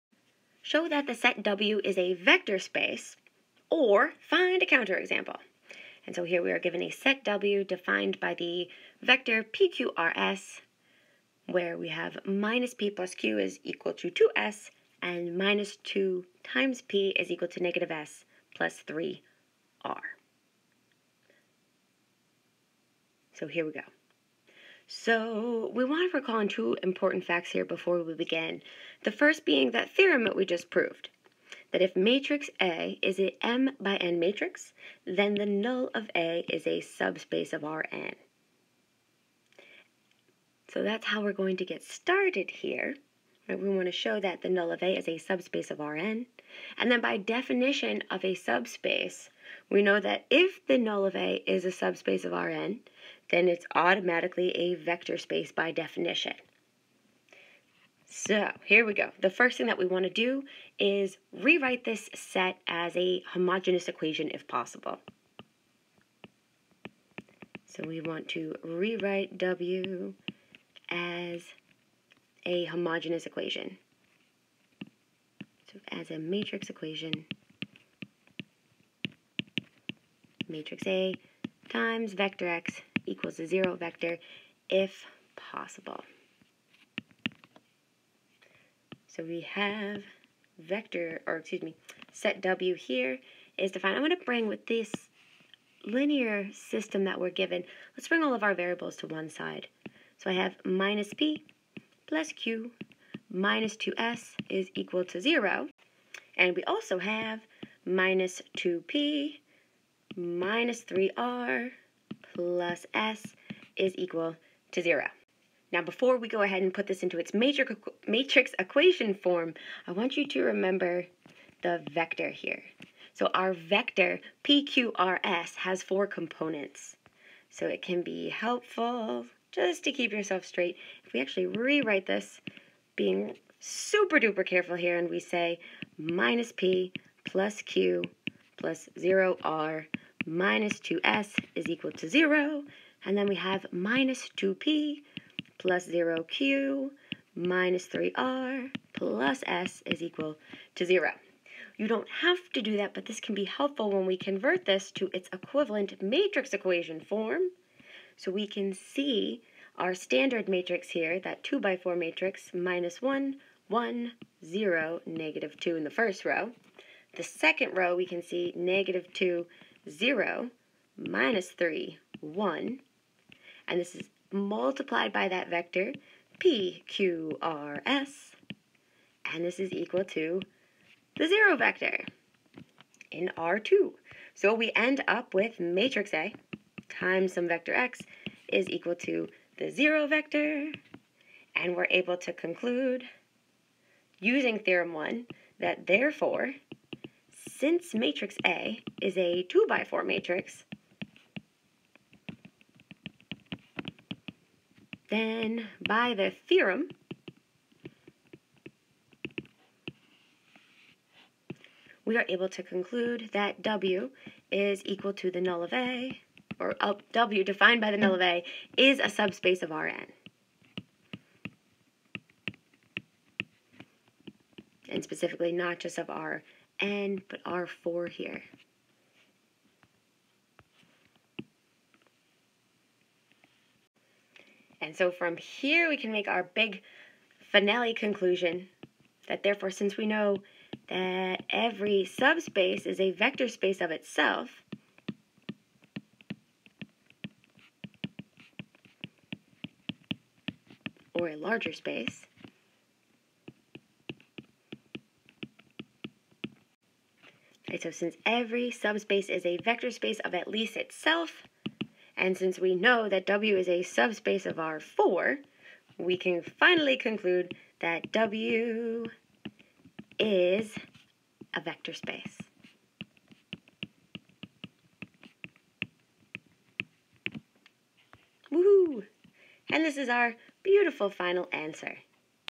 The Null Space of a Matrix (Example 2)
Use the Null Space of a Matrix to Show that W is a Vector Space or Find a Counterexample.